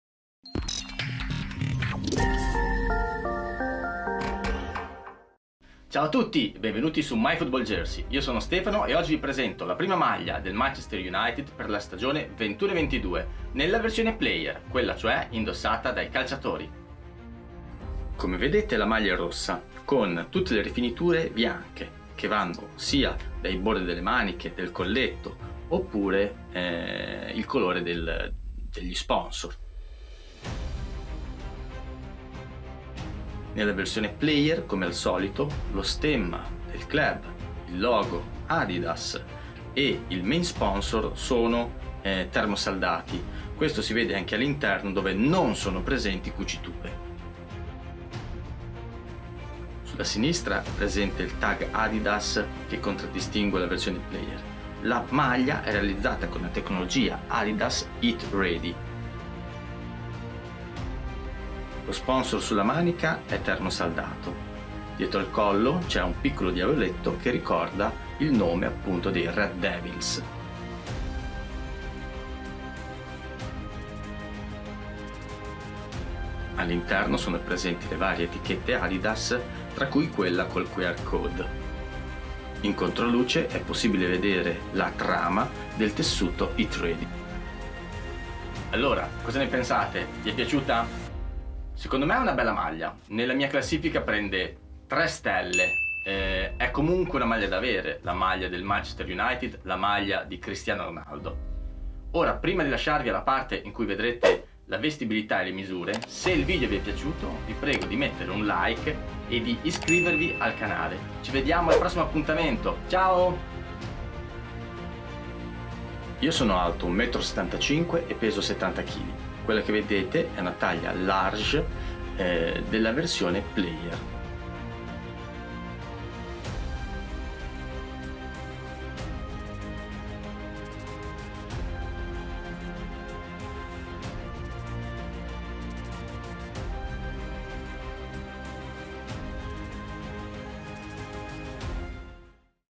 SPtkit 21/22 Manchester United Home Jersey (Player Version) Unboxing Review
🕘 Timecodes
00:06 - Overview
00:32 - Details View
01:35 - Ranking
01:55 – Dimensions & Try ON